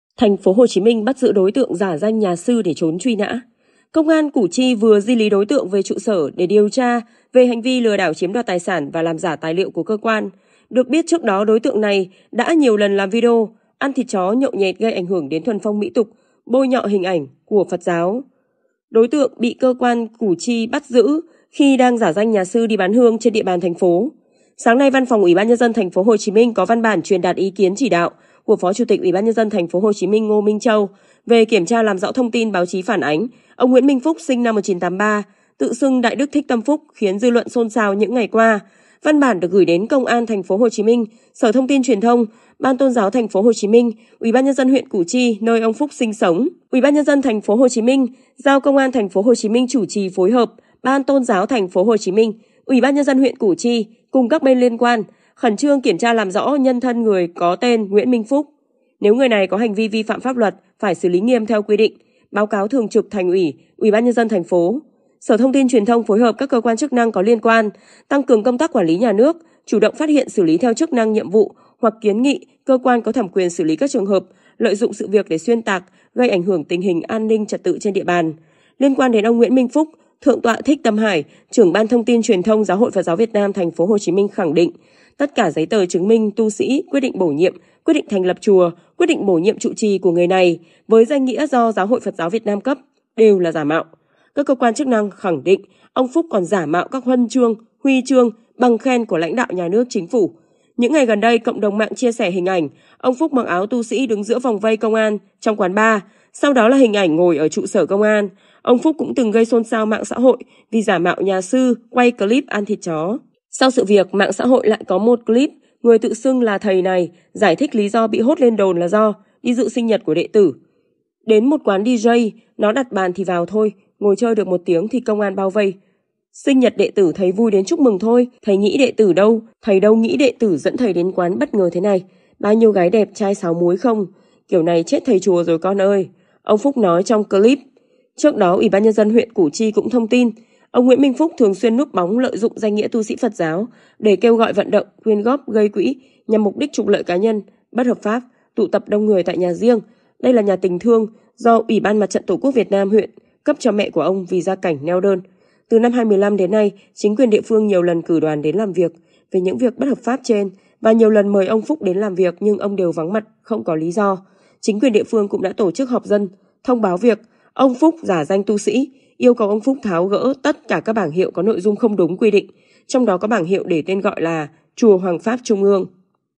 Công an TPHCM bắt giữ nhà sư giả Thích Tâm Phúc sau 13 năm trốn tr,uy n,ã
Công an TPHCM bắt giữ nhà sư giả Thích Tâm Phúc sau 13 năm trốn truy nãThời Sự 24h cập nhật những thông tin hay và hữu ích, những câu chuyện hay xung quanh cuộc sống của chúng ta